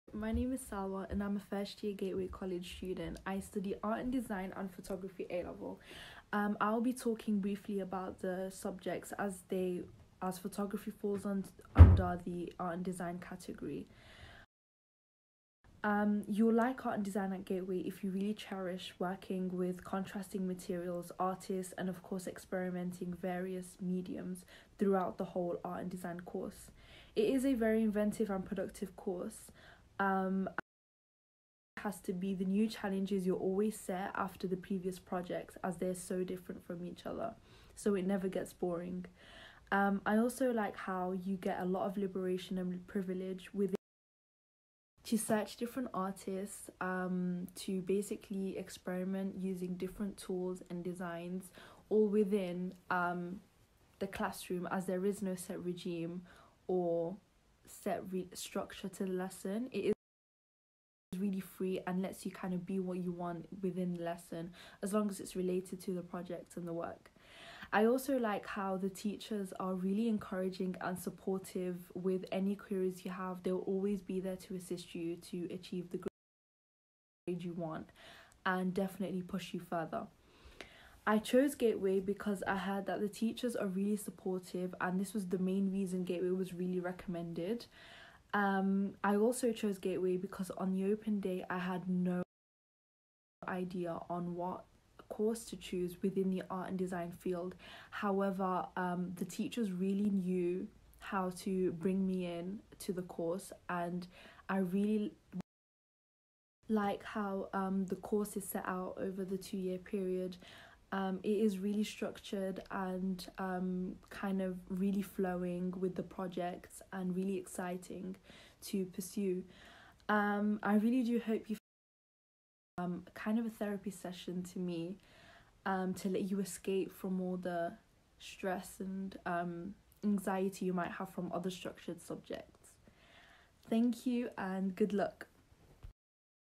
Support | Art and Design | Gateway Welcome Festival 🌠 | Who Will You Become? 🚀
Salwa talks about support in the Art and Design department at Gateway Sixth Form College

Who Will You Become? 🚀 Take one giant leap into your future with Gateway Sixth Form College 🌠